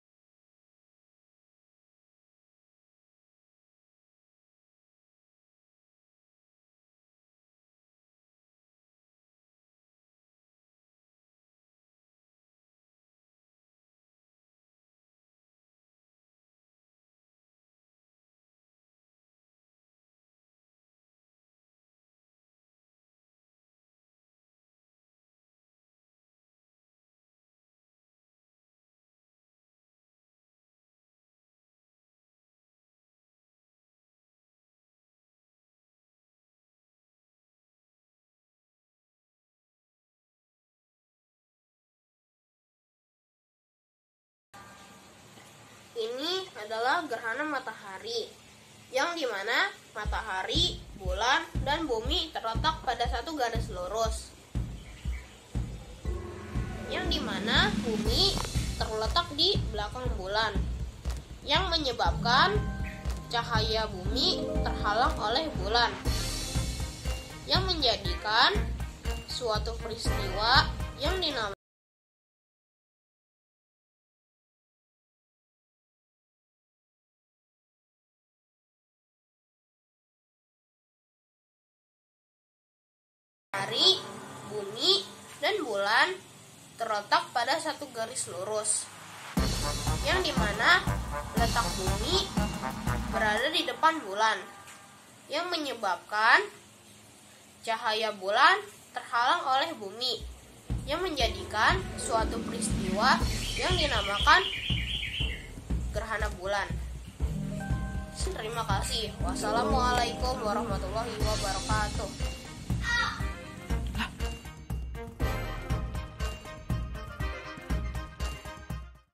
Ujian Praktik IPA Gerhana Matahari dan Gerhana Bulan
Ujian Praktik IPA Kelas 6 Gerhana Matahari dan Gerhana Bulan SDN Pondok Kelapa 01 Jakarta Timur, Tahun Pelajaran 2021/2022